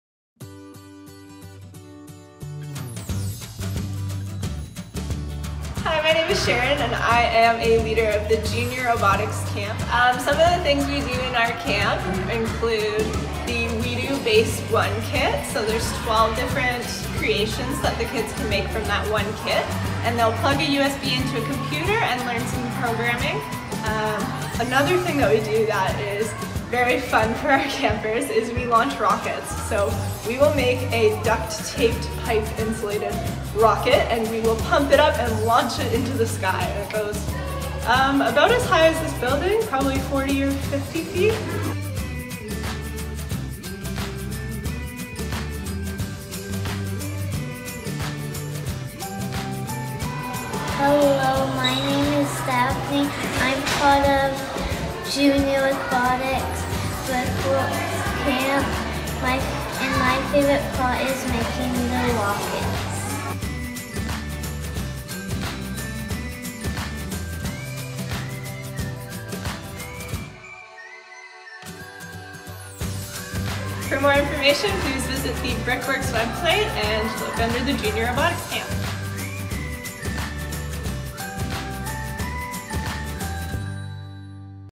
Jr Robotics Overview
Summer Day Camp introducing robotics using Lego and other education based tools